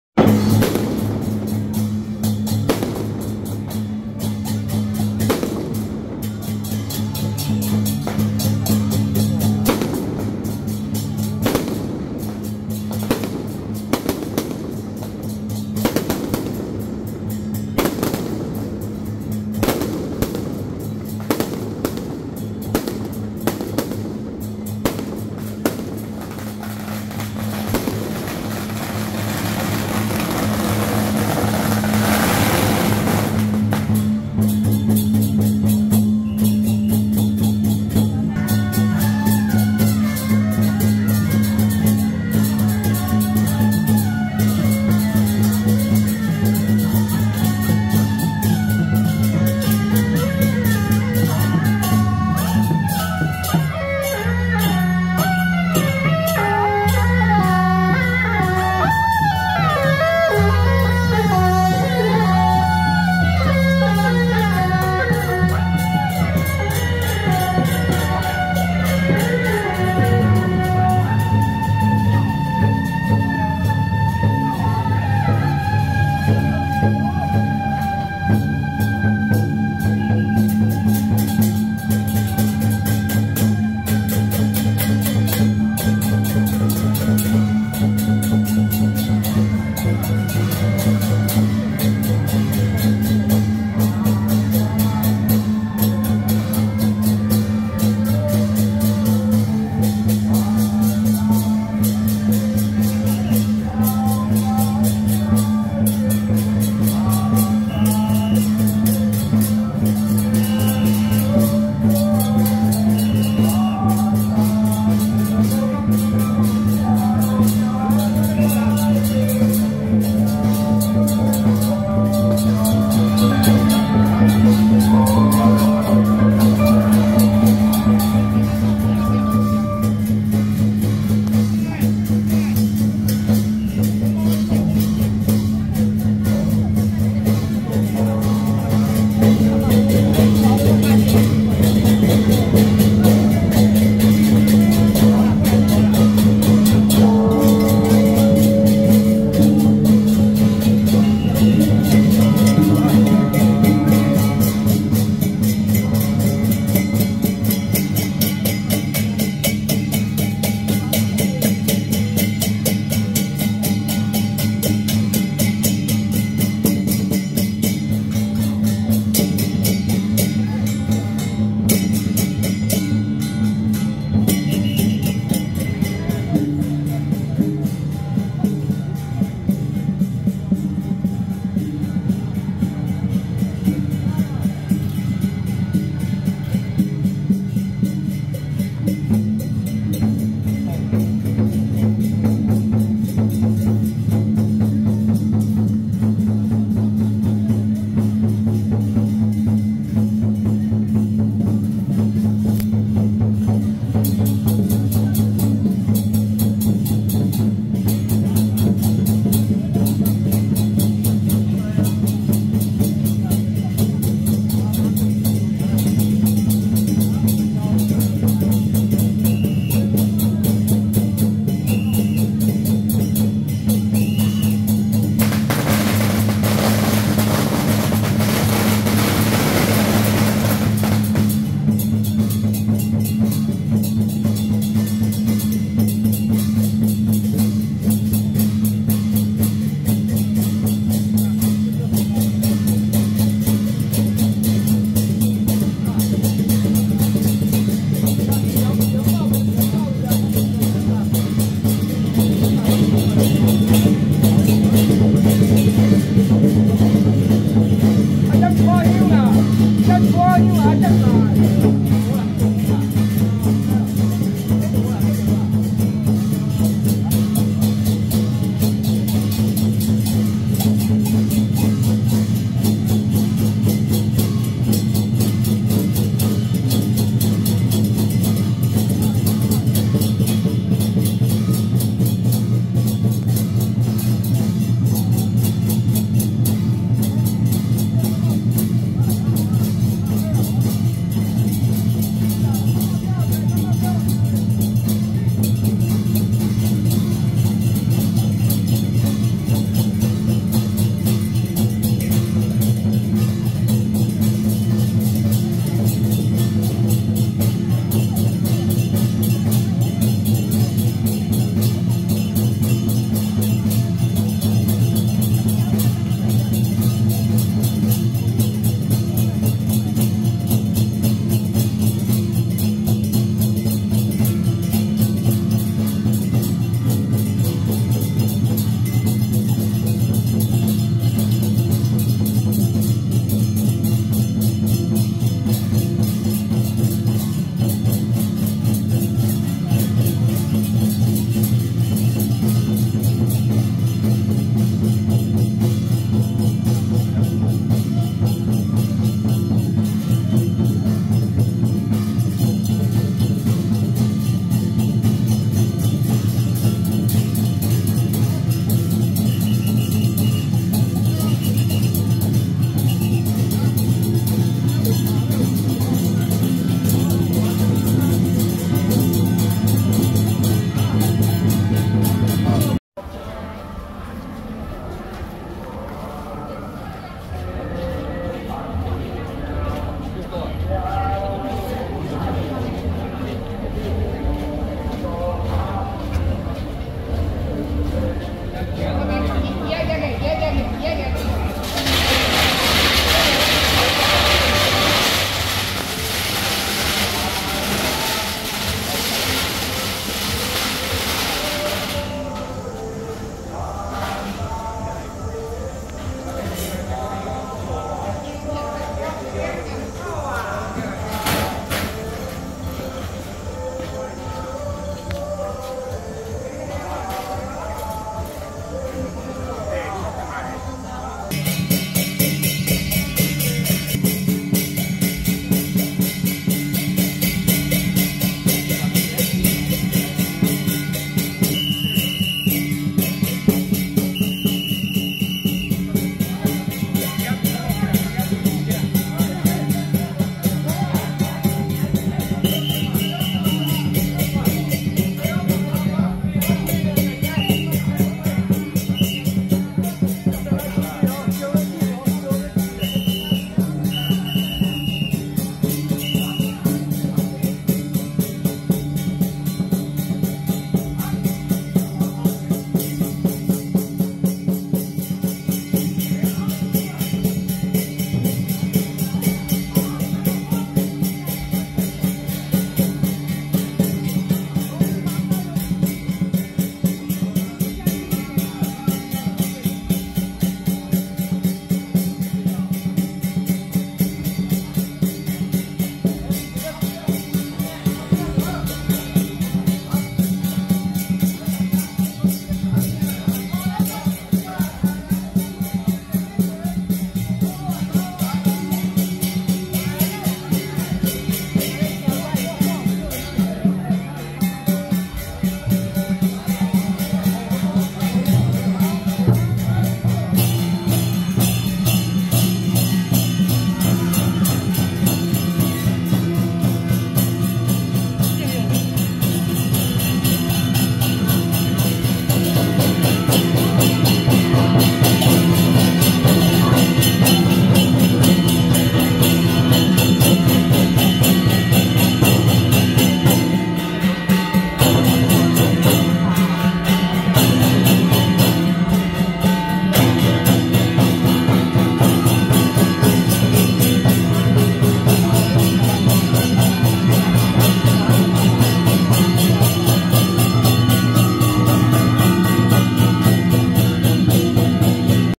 【屏東南州中山王府-開路鼓＆徐府千歲 聖駕(大寮鴻和轎班會)】~112光大寮聚寶宮蕭府太傅北狩大直遶境
屏東南州中山王府-開路鼓＆徐府千歲 聖駕(大寮鴻和轎班會) (0:00)過境 下塔悠鎮安宮 (6:48)入 大直明山寺福天宮